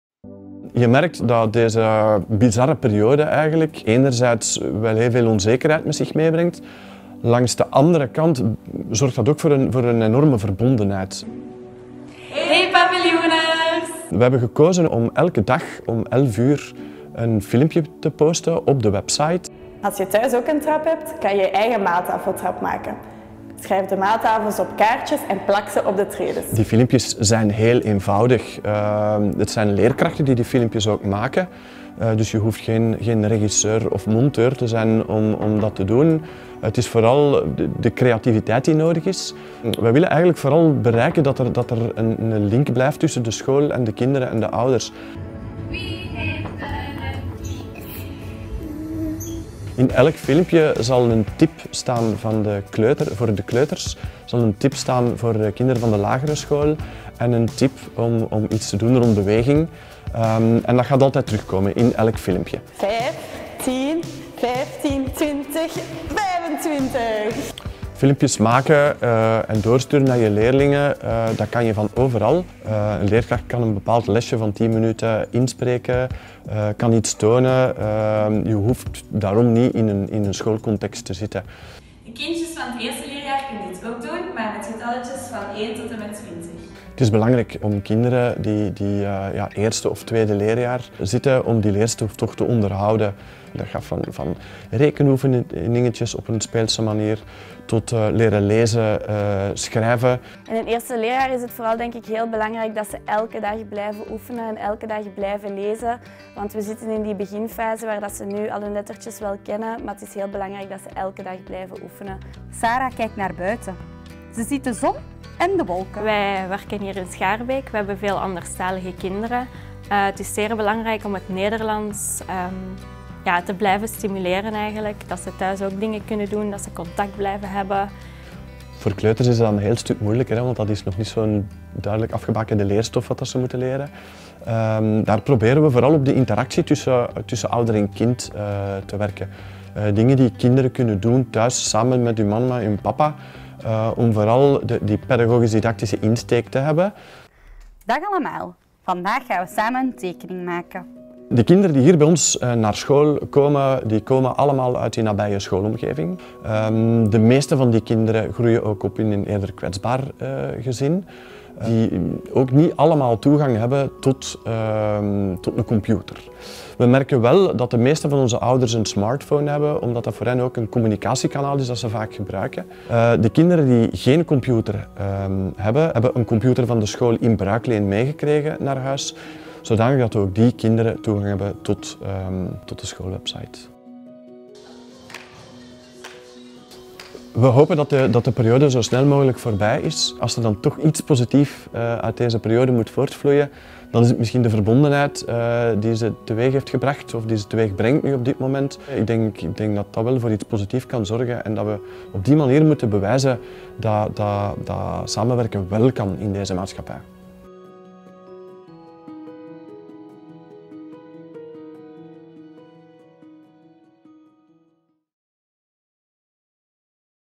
Maak elke dag een filmpje en breng je school tot in de woonkamer
“Mijn leraren sturen hun leerlingen elke dag een zelfgemaakt filmpje’, vertelt Sven, directeur van basisschool Paviljoen in Schaarbeek. “Met tips voor lager én kleuter. Zo houden we contact en zetten we in op leren, ook op afstand.” Kijk mee.